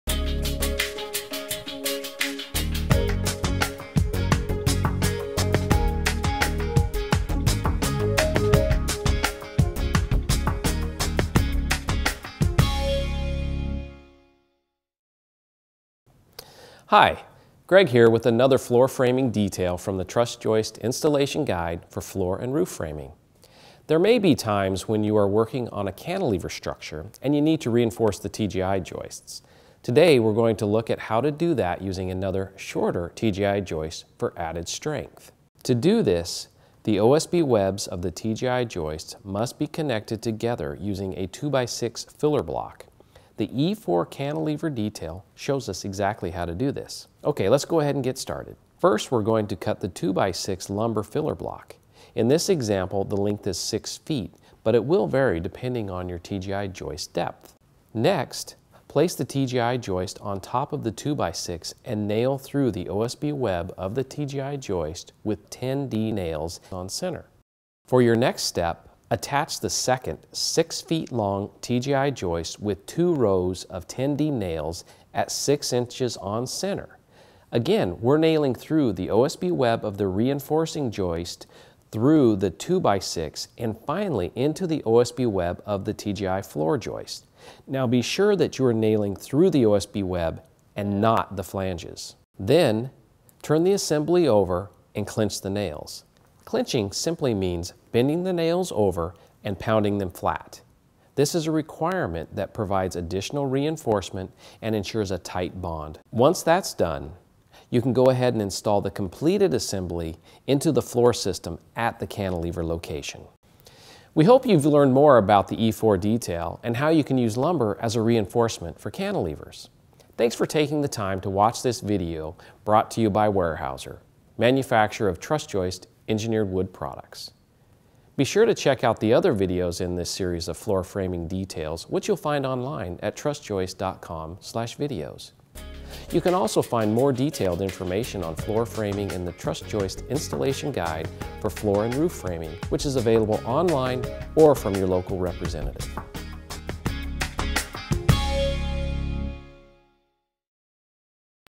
Detail E4 - Trus Joist EWP Floor Installation Guide
How to use a second TJI joist to reinforce the floor joist in a cantilever application.